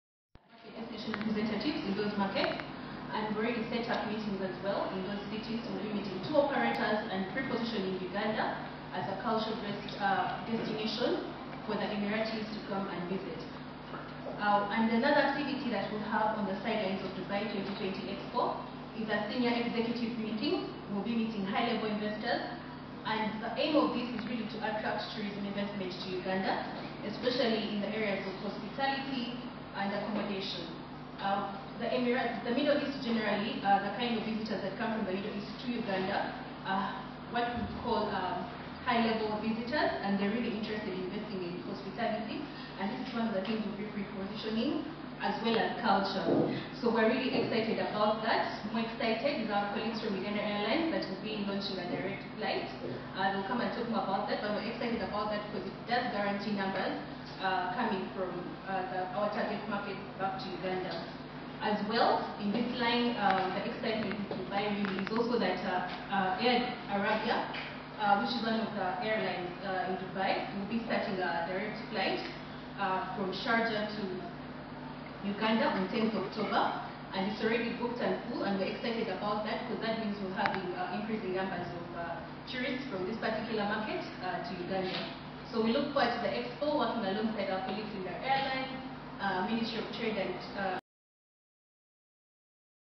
UTB Spokesperson Sandra Natukunda listing what they intend to achieve at the Dubai 2020 Expo
Uganda Tourism Board - UTB Spokesperson Sandra Natukunda listing what they intend to achieve at the Dubai 2020 Expo